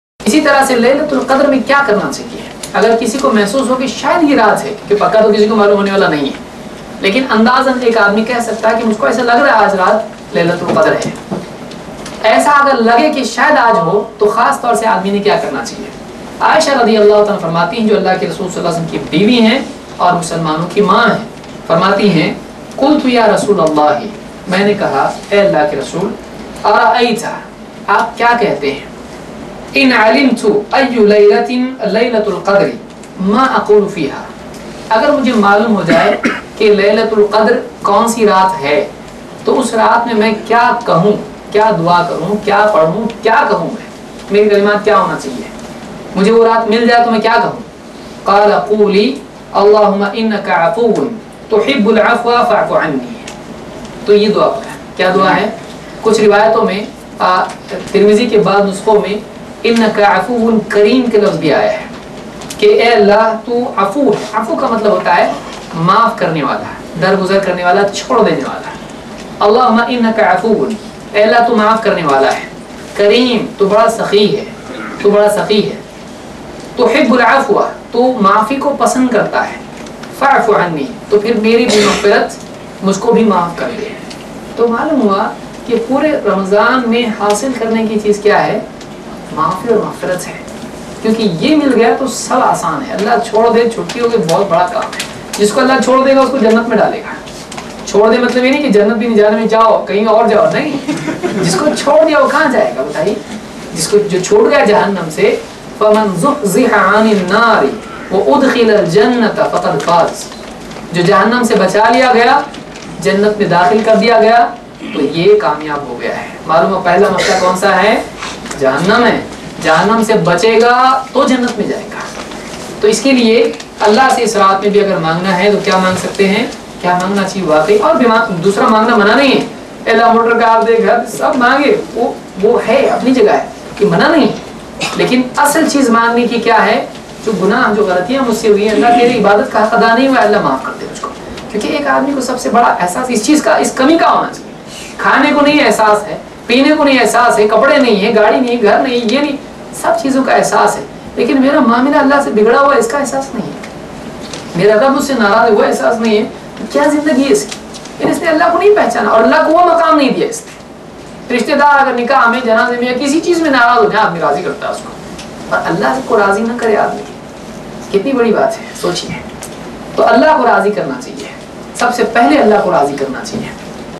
Laillatul Qadr ki Dua | Abu Zaid Zameer
This Video clip is taken from lecture : Ramadhaan kaise Guzarien | Abu Zaid Zameer
Lecture held at :
Hotel Talk About, Phase 1, Rajiv Gandhi Infotech Park,
Hinjewadi, Pune
Date :
18th July 2012.